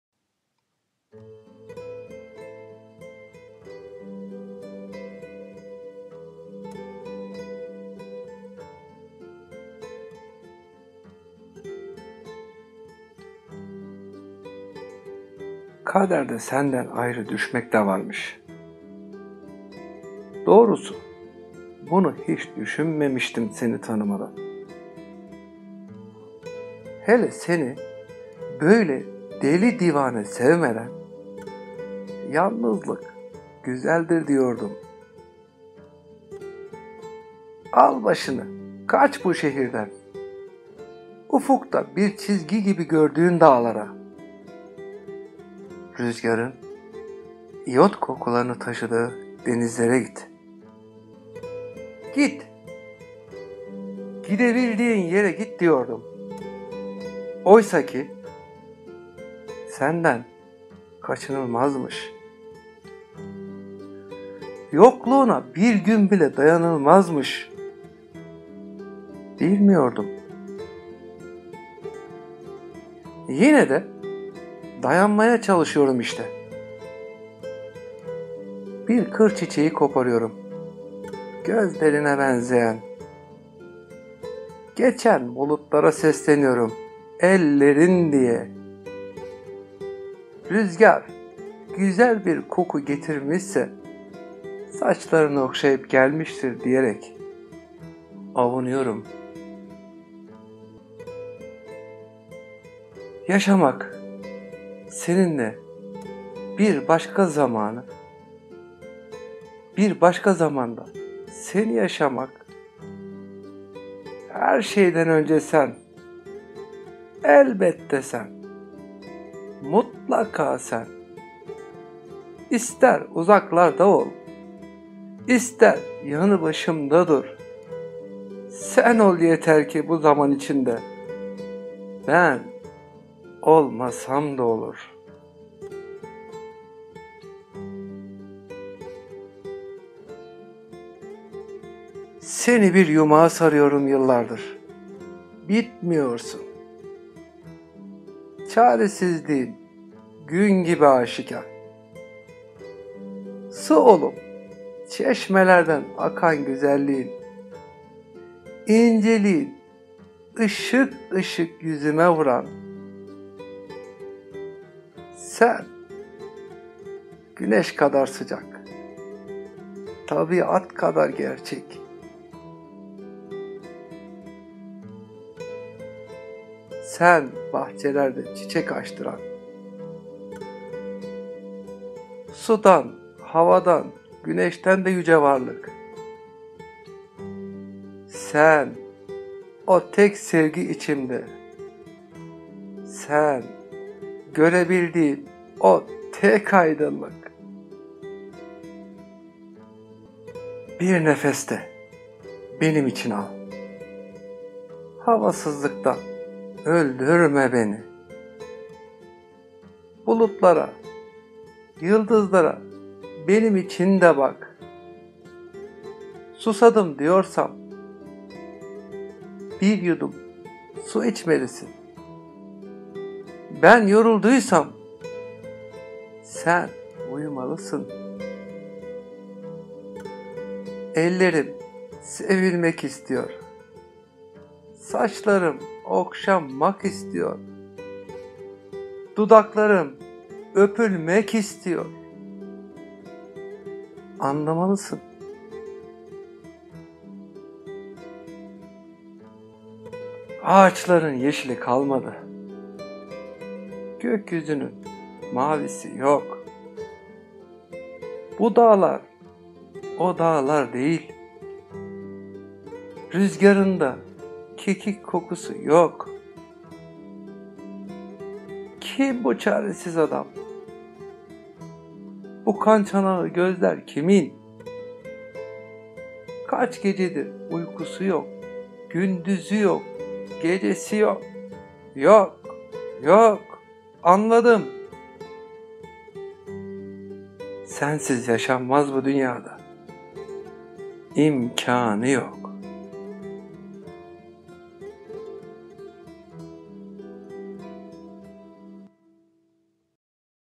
Ümit Yaşar Oğuzcan-Dağ Rüzgarı (Seslendiren:Mustafa Aydoğan)
Kaderde senden ayrı düşmek de varmış
Doğrusu bunu hiç düşünmemiştim...
Seni tanımadan
Hele seni böyle deli divane sevmeden
Yalnızlık güzeldir diyordum
Al başını, kaç bu şehirden
Ufukta bir çizgi gibi gördüğün dağlara
Rüzgarın iyot kokularını taşıdığı denizlere git
Git gidebildiğin yere git diyordum
Oysa ki, senden kaçılmazmış
Yokluğuna birgün bile dayanılmazmış.
Bilmiyordum...

Yine de dayanmağa çalışıyorum işte
Bir kır çiçeği koparıyorum gözlerine benzeyen
Geçen bulutlara sesleniyorum ellerin diye
Rüzgar güzel bir koku getirmişse
Saçlarını okşayıp gelmiştir diyerek avunuyorum
Yaşamak seninle bir başka zamanı
Bir başka zamanda seni yaşamak
Herşeyden önce sen
Elbette sen
Mutlaka sen
İster uzaklarda ol
İster yanıbaşımda dur
Sen ol yeter ki bu zaman içinde
Ben olmasam da olur
Seni bir yumağa sarıyorum yıllardır
Bitmiyorsun
Çaresizliğim gün gibi aşikar
Su olup çeşmelerden akan güzelliğin
İnceliğin ışık ışık yüzüme vuran
Sen güneş kadar sıcak
Tabiat kadar gerçek
Sen bahçelerde çiçekler açtıran
Sudan, havadan, güneşten yüce varlık
Sen, o tek sevgi içimde
Sen görebildiğim tek aydınlık

Bir nefes de benim için al
Havasızlıktan öldürme beni
Bulutlara, yıldızlara benim için de bak
Susadım diyorsam
Bir yudum su içmelisin
Ben yorulduysam sen uyumalısın
Ellerim sevilmek istiyor
Saçlarım okşanmak istiyor
Dudaklarım öpülmek istiyor
Anlamalısın.

Ağaçların yeşili kalmadı
Gökyüzünün mavisi yok
Bu dağlar o dağlar değil
Rüzgarında kekik kokusu yok
Kim bu çaresiz adam
Bu kan çanağı gözler kimin
Kaç gecedir uykusu yok
Gündüzü yok
Gecesi yok
Yok
Yok
Anladım
Sensiz yaşanmaz bu dünyada
İmkanı yok.